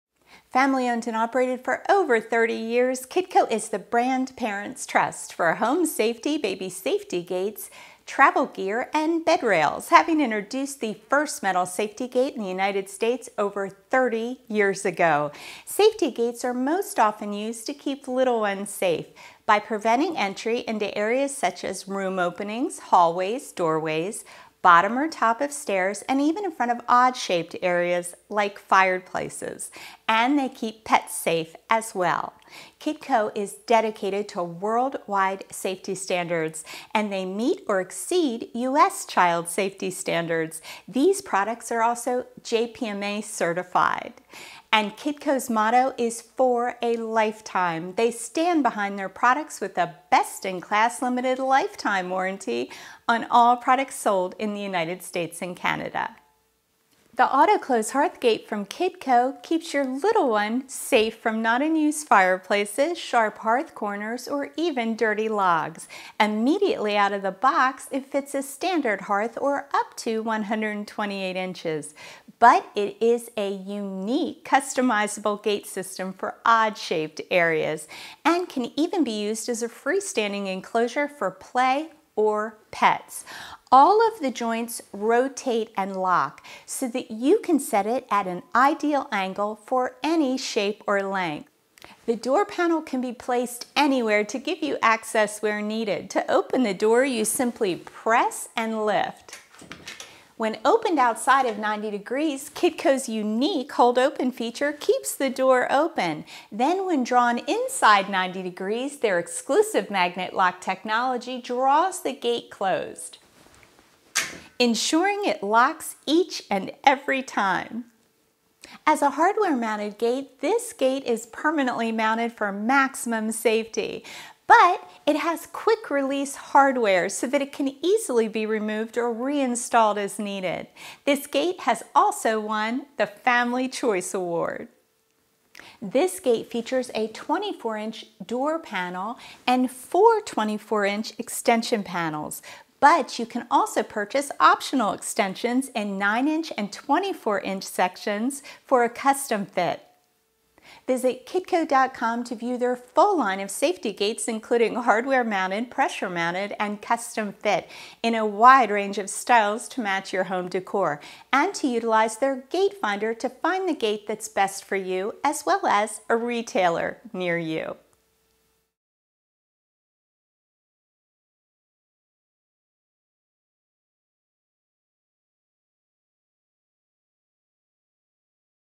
KidCo Auto Close HearthGate Full Overview
This one-of-a-kind gate system fits a standard 6’ hearth and most BBQ grills providing protection from the hot surfaces and flames of in-use fireplaces, accessing gas starter keys, sharp edges, hard corners and dirty logs. The door section can be placed anywhere in the layout giving you access where needed.  Magnet lock technology draws door to the closed position and locks automatically.  Each section is hinged with a rotating joint to allow for custom set up.  Quick release hardware allows for easy removal when needed.  Optional extensions are available in 9” and 24” sections allowing for a custom fit.